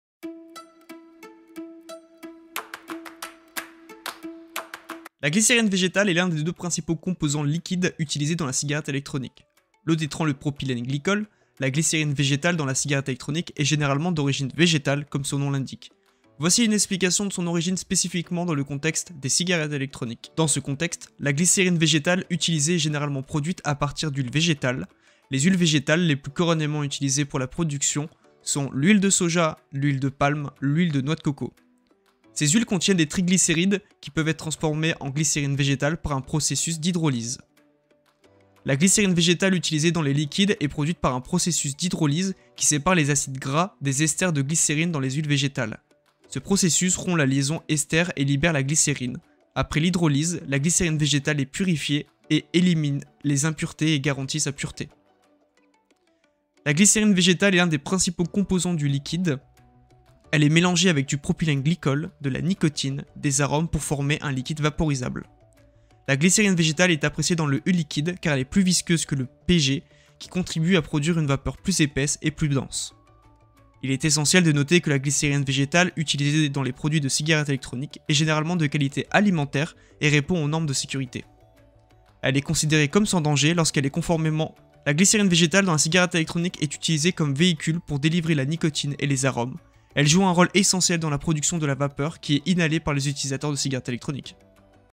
Glycérine Végétale : L'Élément Clé de Votre Vape
( ͡° ͜ʖ ͡°) M E S  R E S E A U X  S O C I A U X( ͡° ͜ʖ ͡°)

••• Instagram : Ma Vape And Diy

Vous êtes-vous déjà demandé ce qui se cache derrière la vapeur douce et savoureuse que vous inhalez lorsque vous utilisez une cigarette électronique ? La réponse réside dans un ingrédient clé : la glycérine végétale (VG). Dans cette vidéo, nous plongeons dans le monde fascinant de la VG et expliquons pourquoi elle joue un rôle essentiel dans votre expérience de vapotage.

Nous commencerons par une introduction à la glycérine végétale, en expliquant son origine, sa composition et comment elle est utilisée dans les e-liquides. Vous découvrirez pourquoi de nombreux vapoteurs préfèrent la VG et comment elle influence la production de vapeur et la saveur.

Ensuite, nous aborderons les aspects liés à la santé et à la sécurité de l'utilisation de la glycérine végétale dans les cigarettes électroniques. Nous discuterons des précautions à prendre et des recommandations pour un vapotage responsable.

Enfin, nous explorerons les différentes utilisations de la VG dans le monde du vapotage, y compris les astuces pour personnaliser votre expérience de vapotage en ajustant le taux de VG dans vos e-liquides.

Si vous êtes un vapoteur expérimenté ou si vous envisagez de passer à la cigarette électronique, cette vidéo vous aidera à mieux comprendre la glycérine végétale et son rôle crucial dans votre expérience de vapotage. N'oubliez pas de cliquer sur la cloche de notification pour être informé des prochaines vidéos.

Prêt à en savoir plus sur la glycérine végétale et à améliorer votre expérience de vapotage ? Alors, commençons !